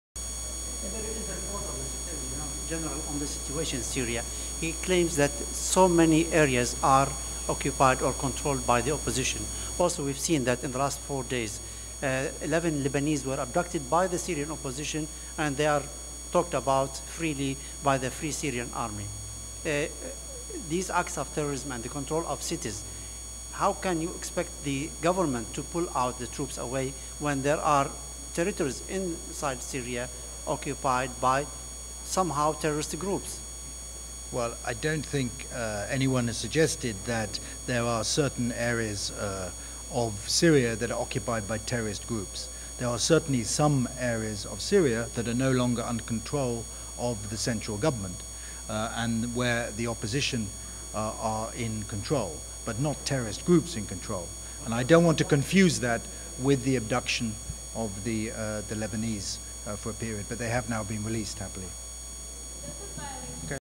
UK, Abducted Lebanese in Syria are they Freed?
UK Sir Mark Lyall Grant Claims the 11 Lebanese abducted by Syrian Rebels are "happily freed"(?)